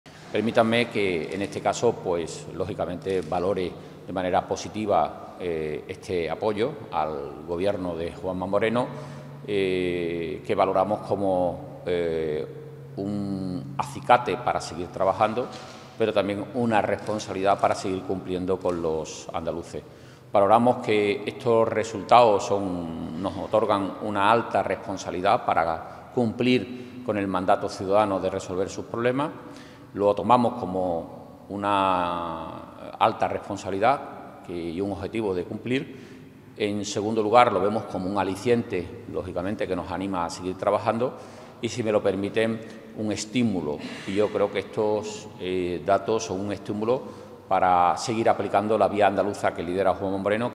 Sanz cree "un estímulo" el sondeo Centra, que "ratifica el apoyo" a Moreno
El consejero de la Presidencia, Interior, Diálogo Social y Simplificación Administrativa, Antonio Sanz, ha asegurado este lunes sobre los datos del Barómetro Andaluz del Centro de Estudios Andaluces (Centra), que vuelve a situar al Partido Popular de Andalucía como primera fuerza de Andalucía con una ventaja de casi 15 puntos sobre el PSOE-A, que se trata de "una ratificación de un apoyo político al Gobierno de Juanma Moreno por el hecho de conservar la mayoría suficiente", cuando la legislatura ha superado su ecuador en Andalucía.